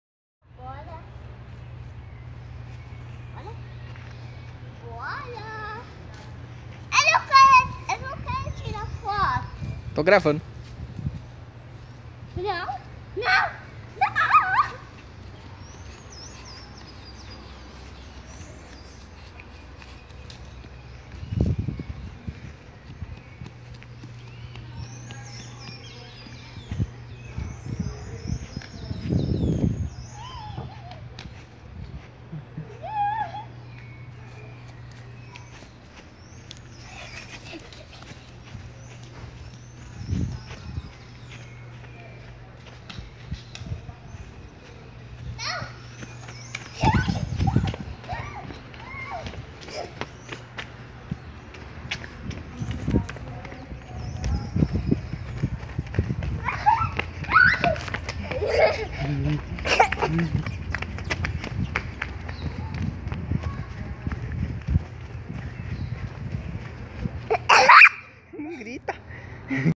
Lorena Brincando na UBS Vila Constância
Atrás da UBS Vila Constância existe um jardim muito lindo. Adorava levar a Nena para passear e brincar nesse espaço.